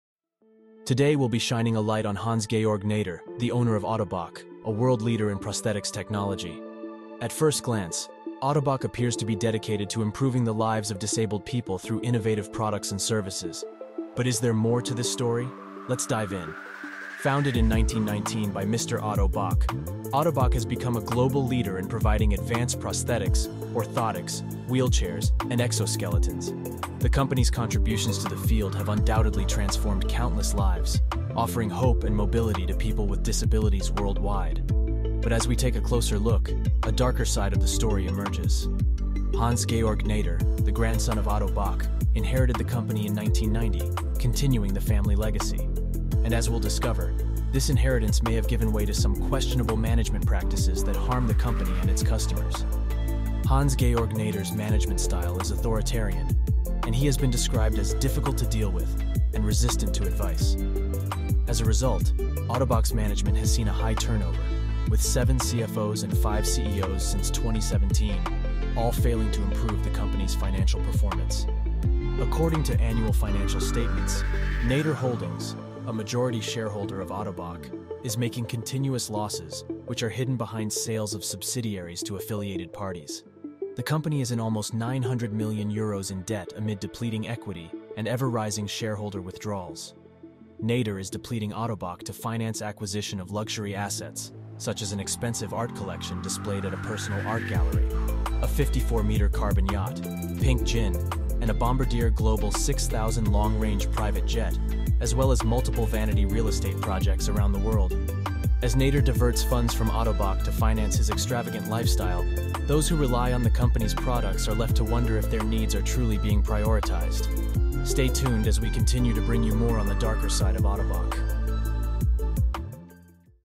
Hans Georg Näder Mismanaging Ottobock
At first glance, Ottobock appears to be dedicated to improving the lives of disabled people through innovative products and services. Founded in 1919 by Mr Otto Bock, Ottobock has become a global leader in providing advanced prosthetics, orthotics, wheelchairs, and exoskeletons.

The company's contributions to the field have undoubtedly transformed countless lives, offering hope and mobility to people with disabilities worldwide. But as we take a closer look, a darker side of the story emerges.

Naeder is depleting Ottobock to finance acquisition of luxury assets such as an expensive art collection displayed at a personal art gallery, a 54-metre carbon yacht - Pink Gin, and a  Bombardier Global 6000 long-range private jet as well as multiple vanity real estate projects around the world.
As Naeder diverts funds from Ottobock to finance his extravagant lifestyle, those who rely on the company's products are left to wonder if their needs are truly being prioritised.